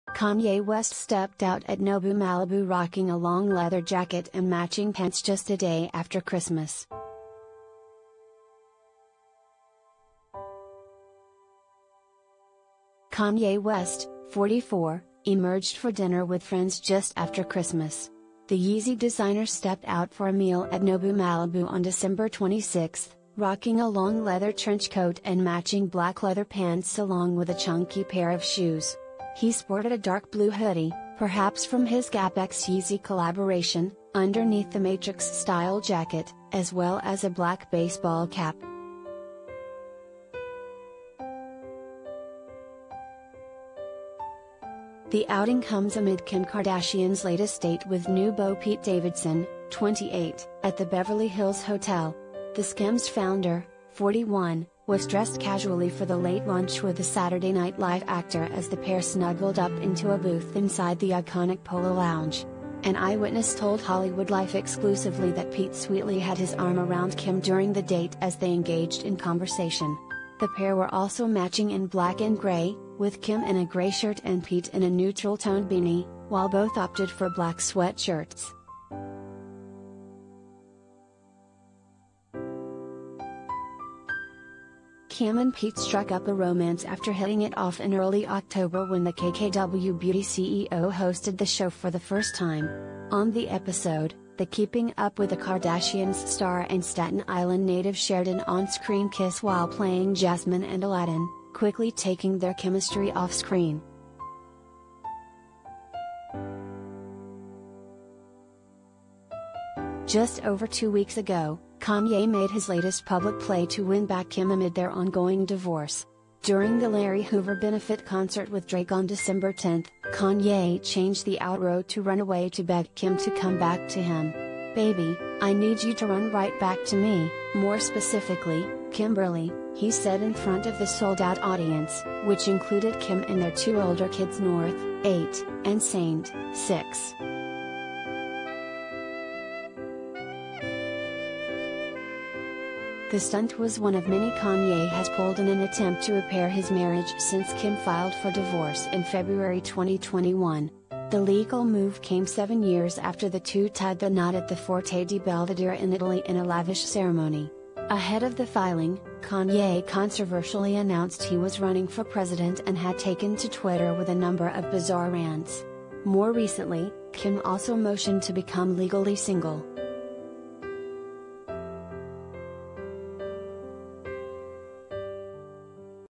Kanye West Spotted With Friends As Kim K & Pete Davidson Step Out For New Romantic Date
Kanye West stepped out at Nobu Malibu rocking a long leather jacket and matching pants just a day after Christmas.
Kanye West, 44, emerged for dinner with friends just after Christmas. The Yeezy designer stepped out for a meal at Nobu Malibu on Dec. 26, rocking a long leather trench coat and matching black leather pants along with a chunky pair of shoes. He sported a dark blue hoodie — perhaps from his Gap x Yeezy collaboration — underneath The Matrix style jacket, as well as a black baseball